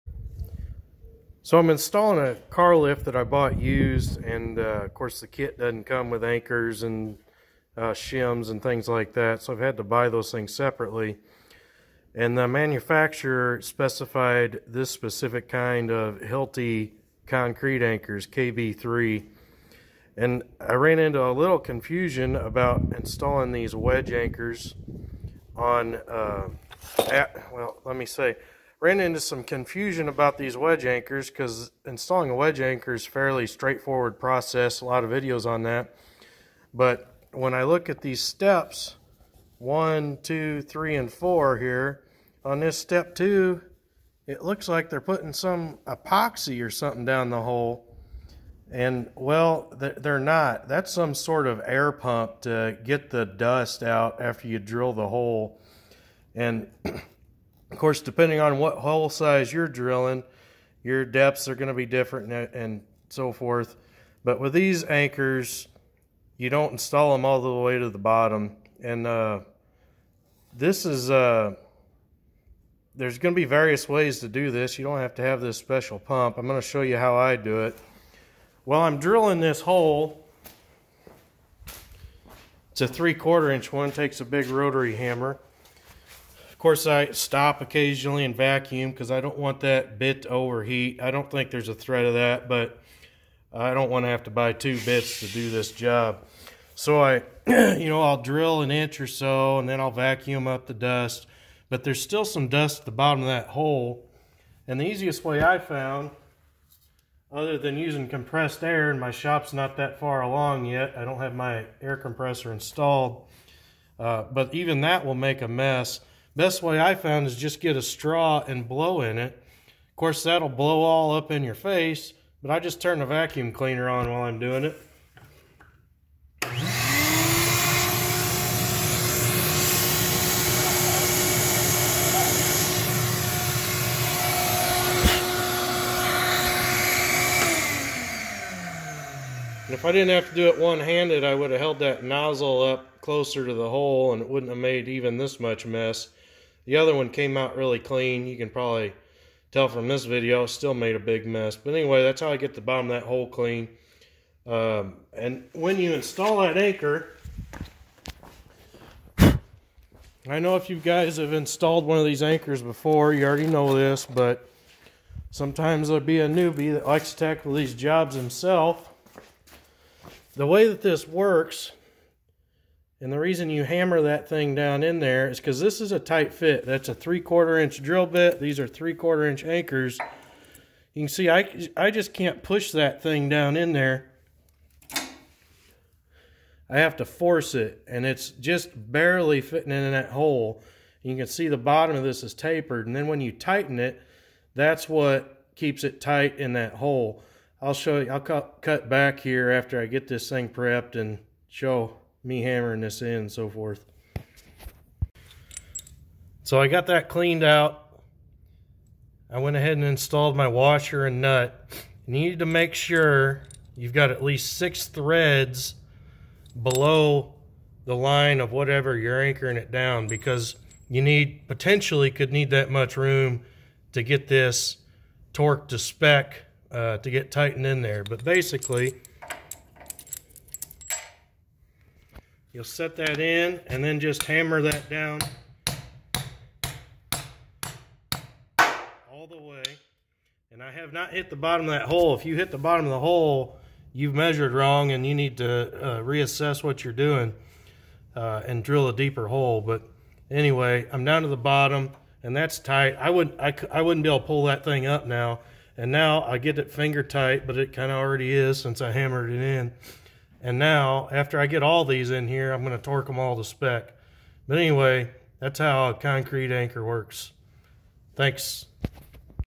Hilti KB3 Wedge Anchor Install
Shows how to install a Hilti KB3 Concrete Wedge Anchor.  Their is one step (Step 2) in the package directions that may be misleading and doesn't seem to be addressed elsewhere.